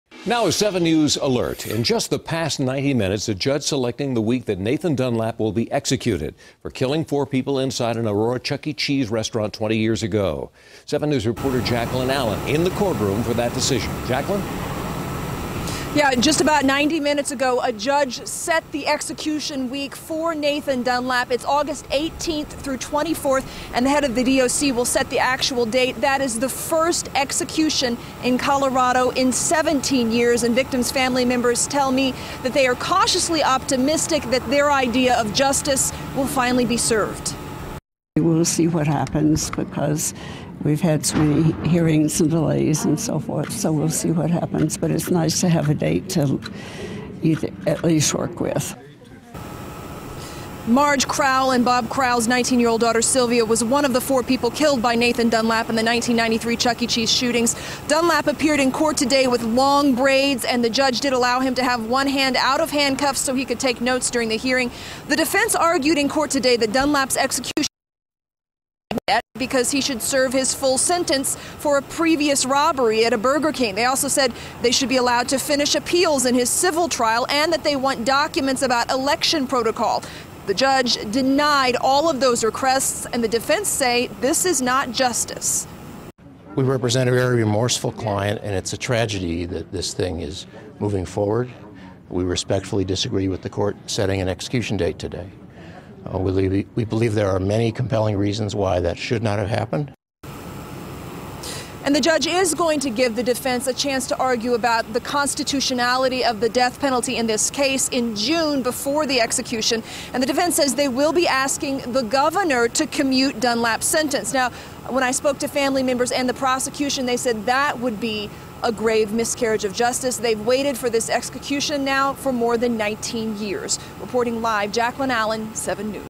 Judge selects window for Dunlap execution
5/1 5 pm -- A judge has decided the execution for condemned killer Nathan Dunlap will take place during the week of August 18 and 24. Jaclyn Allen reports.